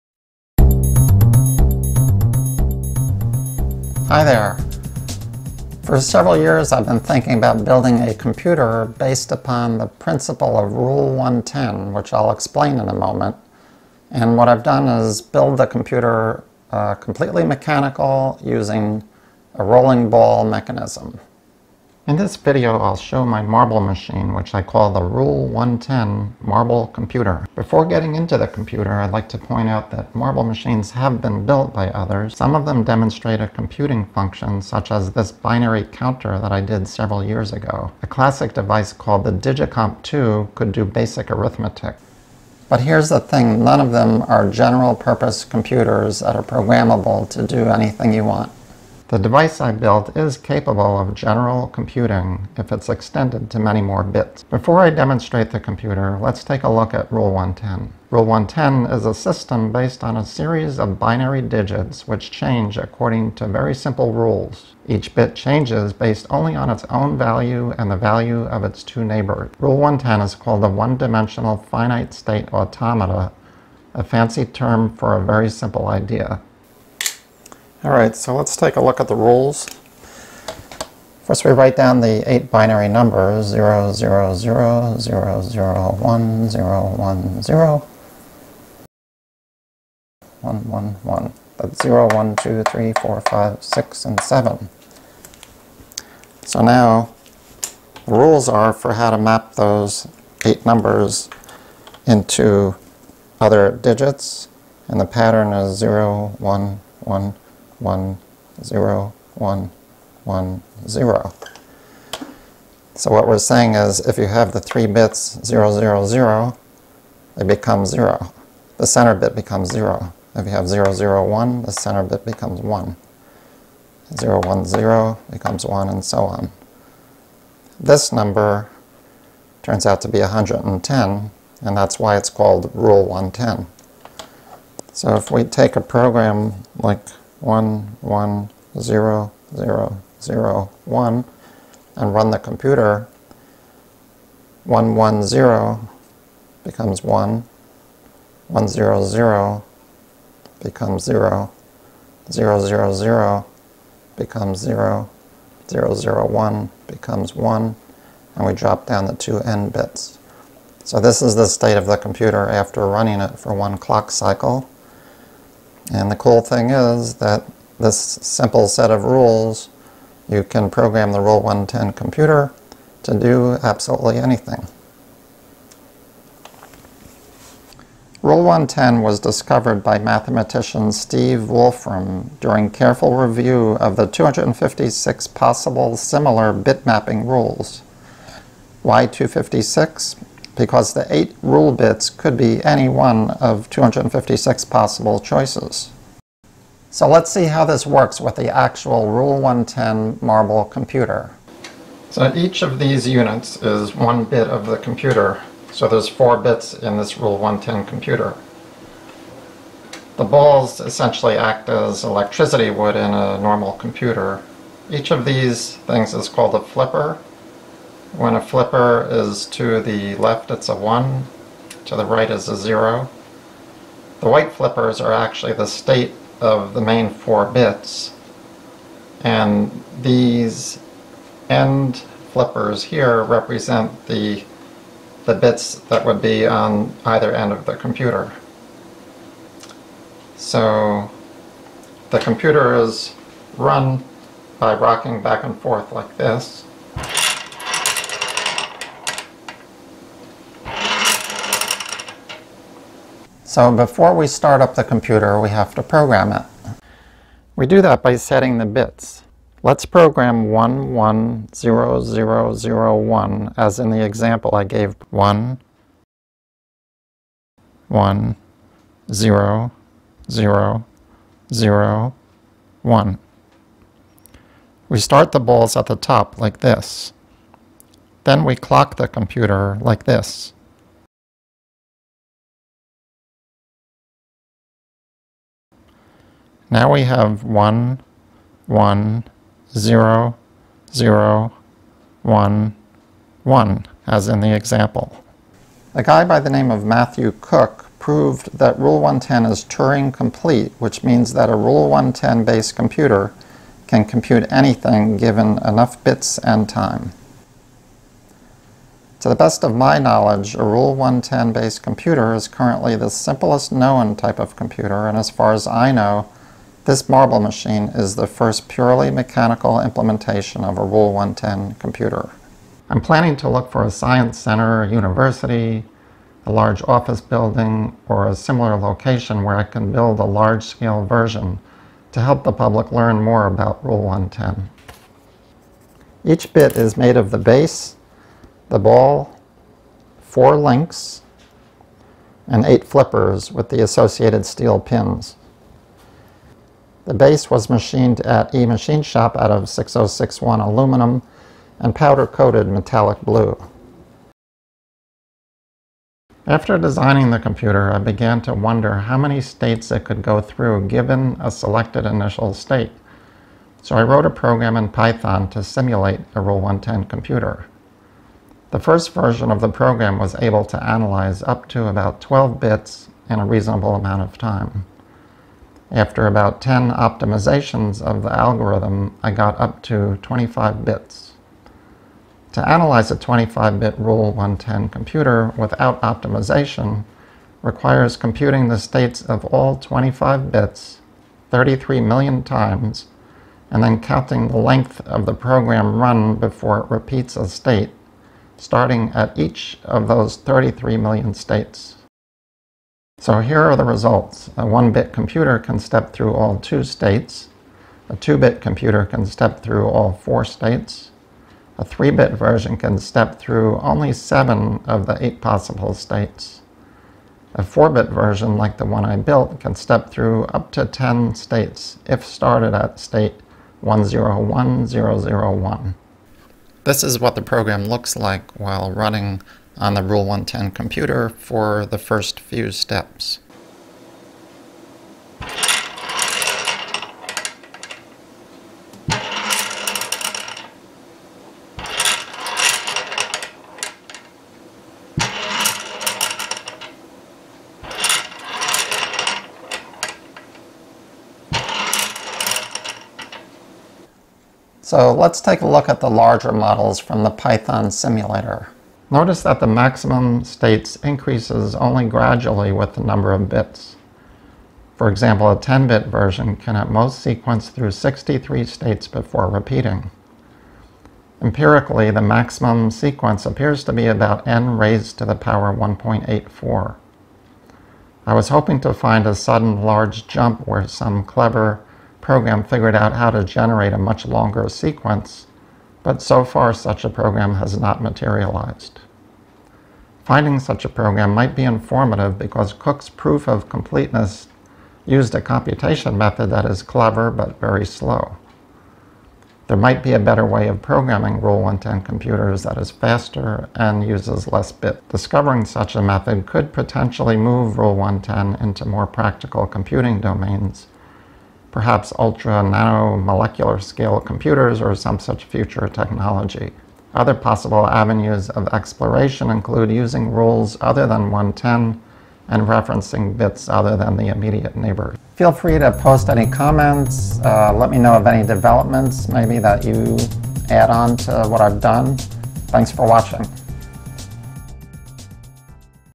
Rule 110 marble computer
Marble based computer by Jim Lewis, CEO of emachineshop.com
Shorter, less technical version:

Transcript:
For several years I've been thinking about building a mechanical computer that demonstrates the Rule 110 principle. In this video I'll show my marble machine which I call the Rule 110 Marble Computer. Many marble machines have been built by others. Some of them demonstrate a computing function such a binary counter I did several years ago. A classic toy called the Digi-Comp II could do basic arithmetic. But none of them are capable of general programmable computing. The device I built is capable of general computing if extended to many more bits. Rule 110 is a system based on a series of binary digits which change according to very simple rules. Each bit changes based only on its own value and the value of its two neighbors. Rule 110 is called a one dimensional finite state automata. Here are the rules. If there are three zeros the center bit changes to 0. 001 becomes 1. 010 becomes 1. 011 becomes 1. 100 becomes 0. 101 becomes 1. 110 becomes 1. And 111 becomes 0.  So let's say we start with 110001. That's our program. We take the bits in groups of 3 and apply the rule. So 110 becomes 1. 100 becomes 0. 000 becomes 0 and 001 becomes 1. In my computer I'm leaving the end bits fixed so we drop them down. So the new pattern is 110011. The way the rule 110 computer runs is by updating the bits repeatedly.  But what's cool is that with enough bits you can program a rule 110 machine to do anything. Rule 110 was discovered by mathematician Steve Wolfram during careful review of the 256 possible similar bit-mapping rules. Why 256? Because the 8 rule bits could be any of 256 possible choices. So let's see how all this works in the marble computer. The balls act as electricity would in a normal computer. When pointing to the left each flipper is a 1 and to the right is a 0. The white flippers represent the state of main four units and these end flippers represent the bits at either end of the computer. The computer is run by rocking back and forth. Before we start up the computer, we have to program it. We do that by setting the bits. Such as 1,1,0,0,0,1 as in the example I gave previously. Matthew Cook proved that Rule 110 is Turing complete which means that a rule 110 based computer can compute anything given enough bits and time. To the best of my knowledge a rule 110 based computer is currently the simplest known type of computer and as far as I know, this marble machine is the first purely mechanical implementation of a rule 110 computer. As far as future plans, I'm planning to look for a science center, university or another public location where I can build a large scale version to help the public learn more about Rule 110. Each bit is made of the base, the ball, 4 links and 8 flippers. The base was machined at eMachineShop out of 6061 aluminum and powder coated metallic blue. After I designed the computer I began to wonder how many states it could go thru given a selected initial state. So I wrote a program in python to simulate a rule 110 computer. The first version of the program was able to analyze up to about 12 bits in a reasonable amount of time. After about 10 optimizations of the algorithm I got up to 25 bits. To analyze a 25 bit rule 110 computer without optimization requires computing the states of all 25 bits, 33 million times and then counting the length of the program run before it repeats a state, starting at each of those 33 million states. So here are the results. A one bit computer can step thru all 2 states. A 2 bit computer can step thru all 4 states. A 3 bit version can step thru only 7 of the 8 possible states. A 4 bit version like the one I built can step thru up to 10 states … if started at state 101001. As for larger machines, the maximum states increases only gradually with the number of bits. For example a 10 bit version can at most sequence thru 63 states before repeating. Empirically the maximum sequence appears to be about N raised to the power 1.84. I was hoping to find a sudden large jump where some clever program figured out how to generate a much longer sequence but so far such a program has not materialized. Finding such a program might be informative because Cook's proof of completeness used a computation method that is clever but very slow. There might be a better way of programming rule 110 computers that is faster and uses less bits. Discovering such a method could potentially move rule 110 into more practical computing domains - perhaps ultra nano molecular scale computers or some such future technology. Other possible avenues of exploration include using rules other than 110 and referencing bits other than the immediate neighbors.